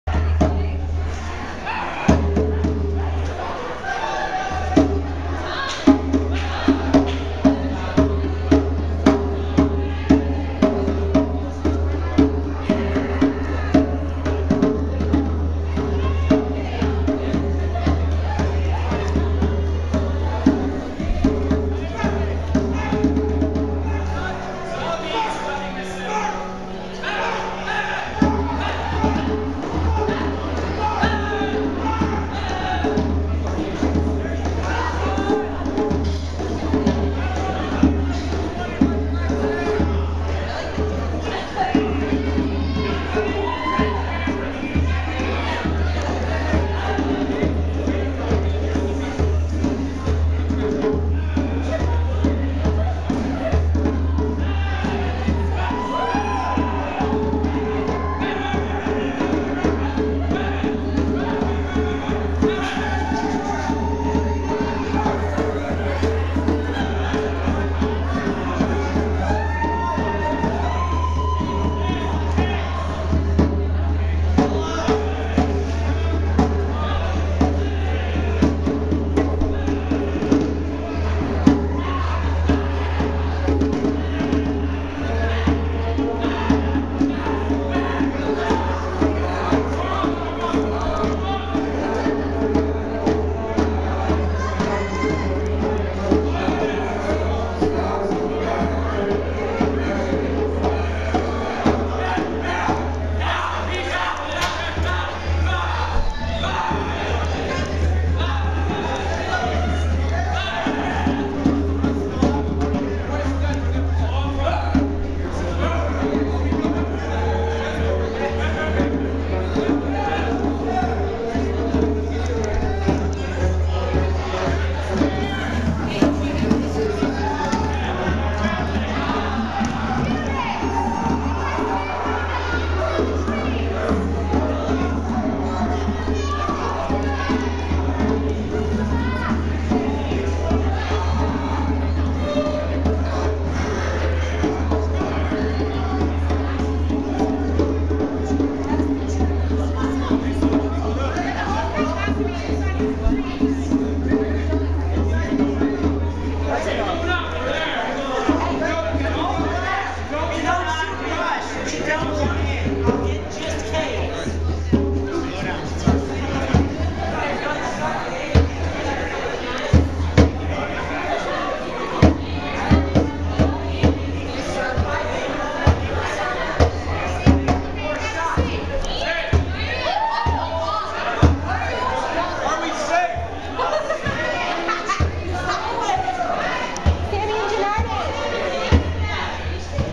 Humans Successfully Reach Tuesday Night's Checkpoint
Humans vs. Zombies game at New College of Florida

New College Zombies 2010